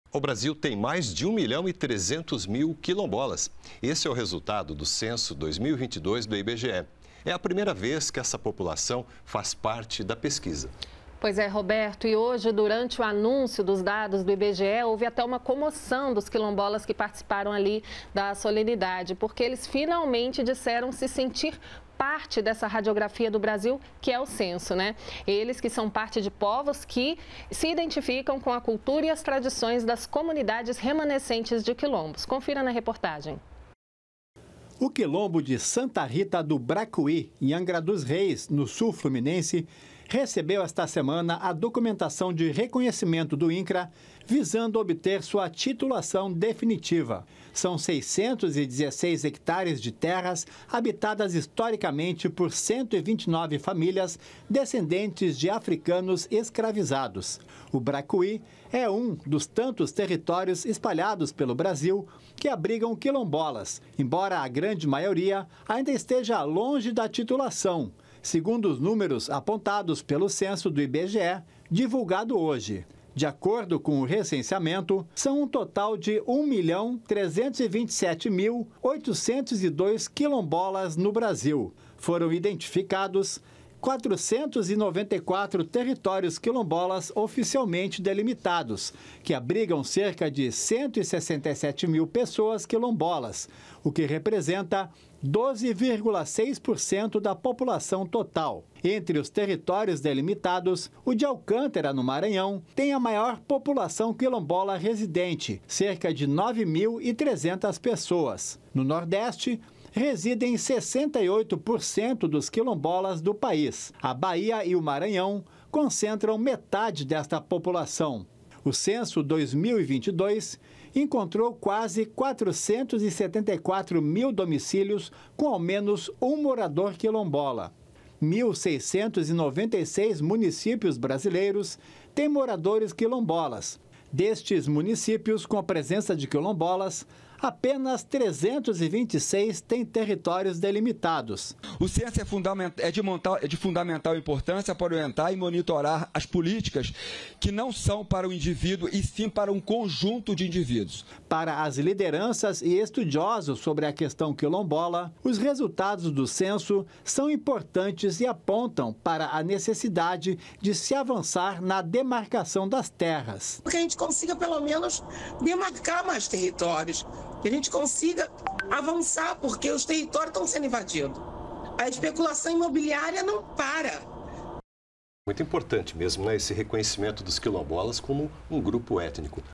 BRASIL TEM 1,3 MILHÃO DE QUILOMBOLAS
O Brasil tem mais de 1,3 milhão de quilombolas. Esse é o resultado do Censo 2022 do IBGE. É a primeira vez que essa população faz parte da pesquisa.